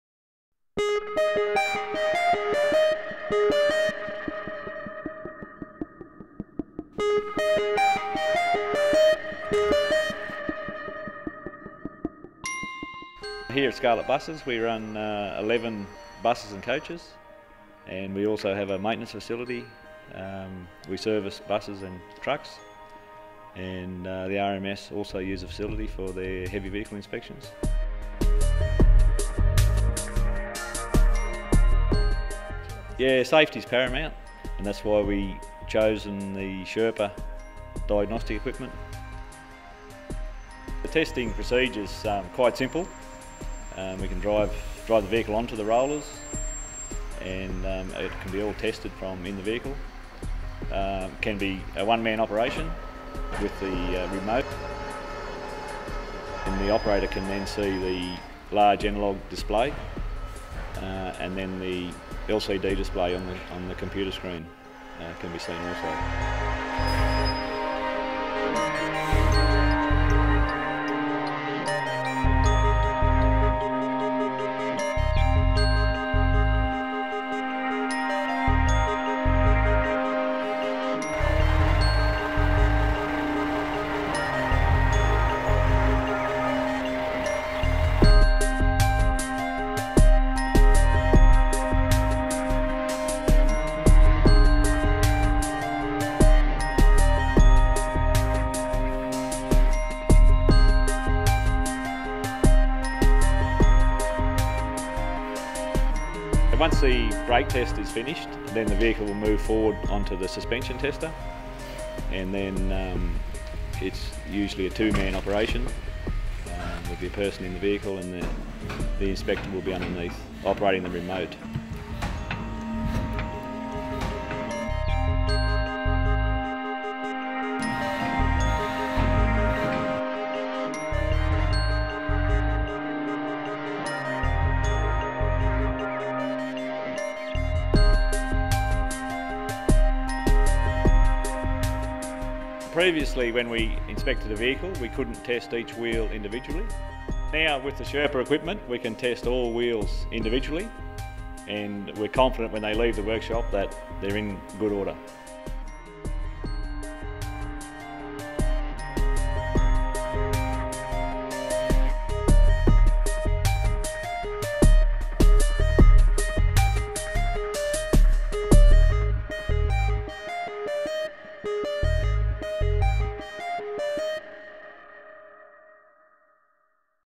Bremsenprüfung bei einem LKW mit dem Bremsenprüfstand und Achsspieltester vom Sherpa
Der LKW Bremsenprüfstand von Sherpa im Praxistest.

Wulf Werkstattausrüstung GmbH
Individuelle Beratung für noch effizienteres Arbeiten in Ihrer KFZ- Werkstatt!